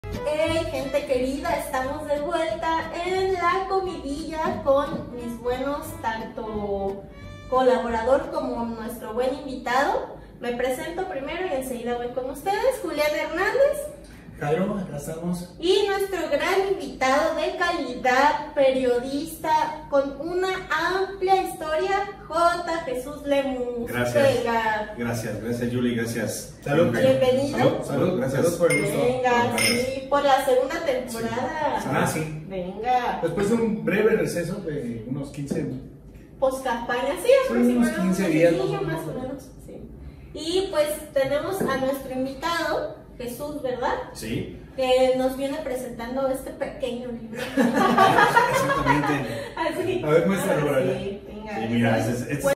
LaComidilla
LaComidilla 🍽La Comidilla - Episodio 8 - Invitado J. Jesús Lemus 🍴

Julliana y Jairo nos preparan unas deliciosas hamburguesas con la ayuda del escritor y periodista J. Jesús Lemus.